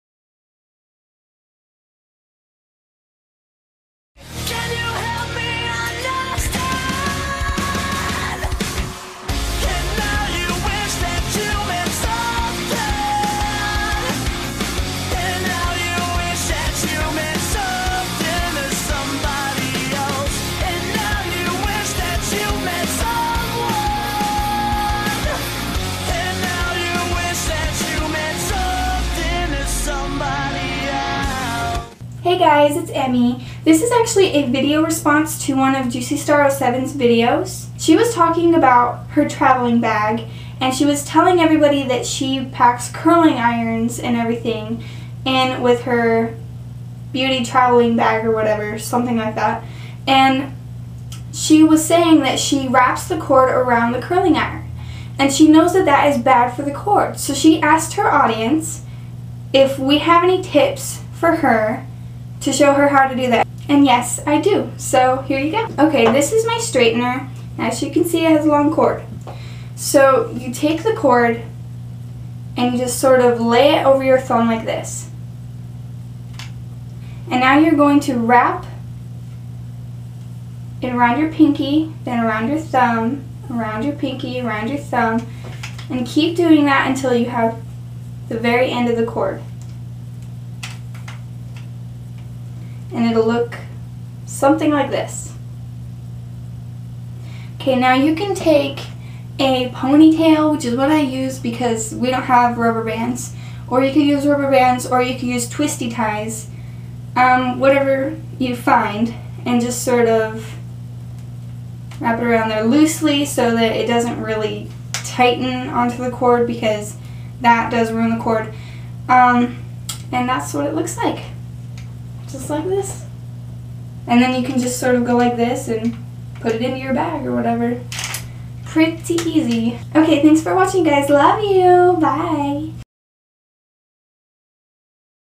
Howto: ♥Coil Cords on Curling Irons/Others for Storing!♥ :)
This is a video response to one of Juicystar07's videos. :)
Hope this helps you, Blair! ;)